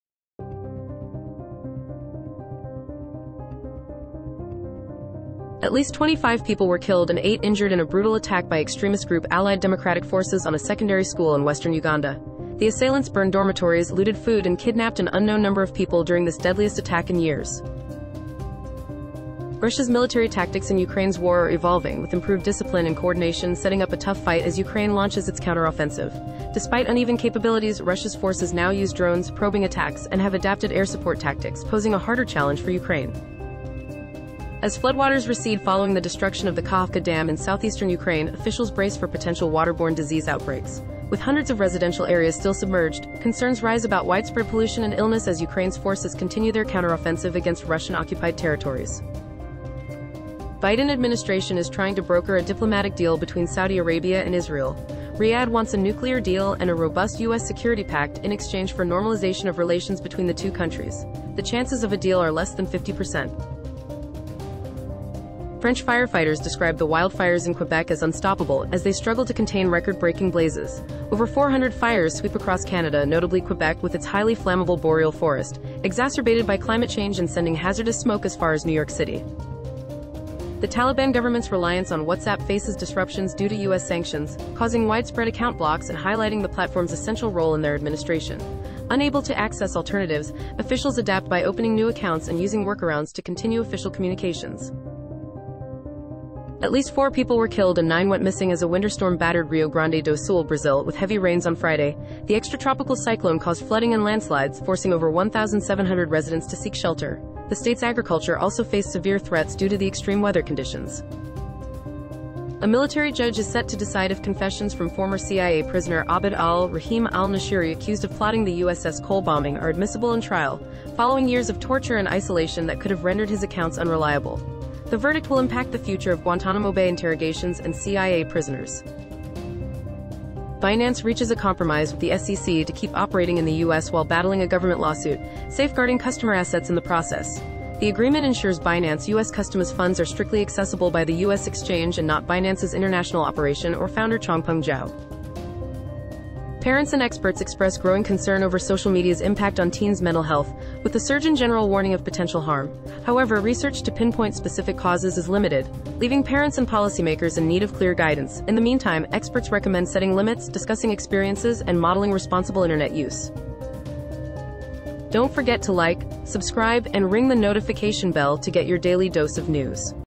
Top 10 News from June 18, 2023
00:05 - 1. At least 25 people were killed and eight injured in a brutal attack by the extremist group Allied Democratic Forces on a secondary school in western Uganda...
00:18 - 2. Russia's military tactics in Ukraine's war are evolving, with improved discipline and coordination, setting up a tough fight as Ukraine launches its counteroffensive...
00:32 - 3. As floodwaters recede following the destruction of the Kakhovka dam in southeastern Ukraine, officials brace for potential waterborne disease outbreaks...
00:47 - 4. Biden administration is trying to broker a diplomatic deal between Saudi Arabia and Israel...
01:01 - 5. French firefighters describe the wildfires in Quebec as "unstoppable," as they struggle to contain record-breaking blazes...
01:15 - 6. The Taliban government's reliance on WhatsApp faces disruptions due to US sanctions, causing widespread account blocks and highlighting the platform's essential role in their administration...
01:30 - 7. At least four people were killed and nine went missing as a winter storm battered Rio Grande do Sul, Brazil, with heavy rains on Friday...
01:45 - 8. A military judge is set to decide if confessions from former CIA prisoner Abd al-Rahim al-Nashiri, accused of plotting the USS Cole bombing, are admissible in trial, following years of torture and isolation that could have rendered his accounts unreliable...
02:02 - 9. Binance reaches a compromise with the SEC to keep operating in the US while battling a government lawsuit, safeguarding customer assets in the process...
02:17 - 10. Parents and experts express growing concern over social media's impact on teens' mental health, with the surgeon general warning of potential harm...